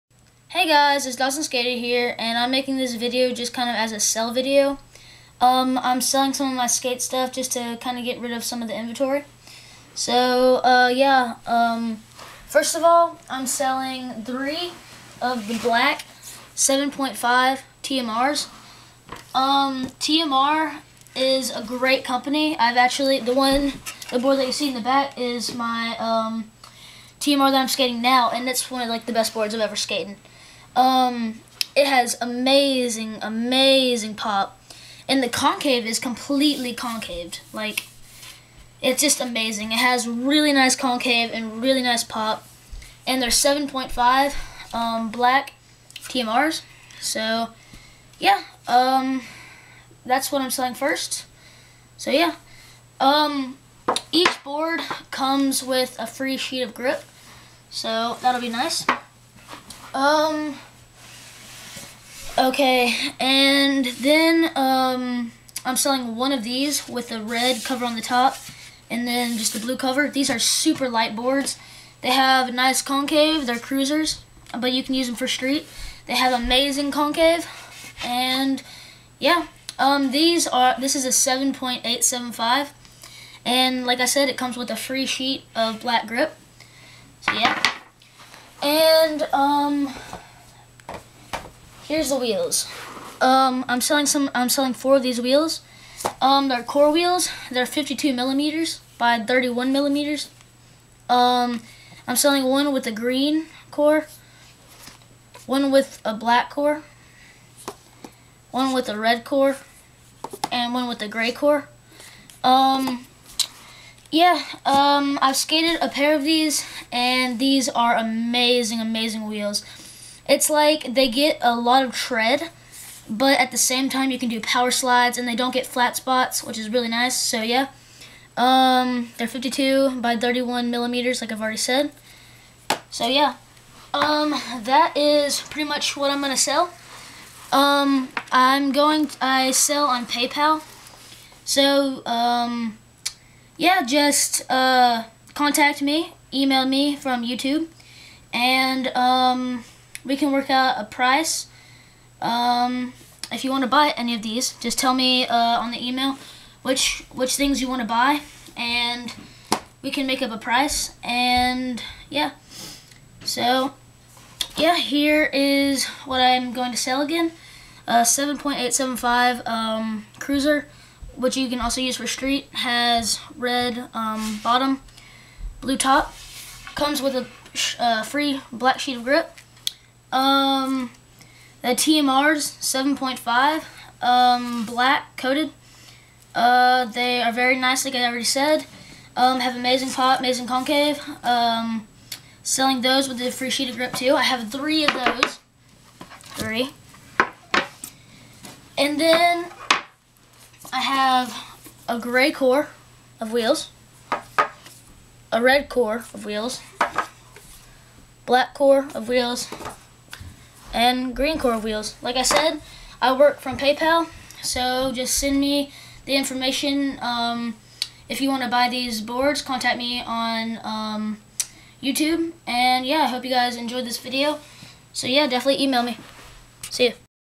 Selling Skate Stuff
Hello i am selling some of my skate stuff. i have 3 TMR 7.5 decks and one cruiser/street 7.875 and all of them come with a free sheet of grip tape. I also am selling 52 mm wheels with different colored cores.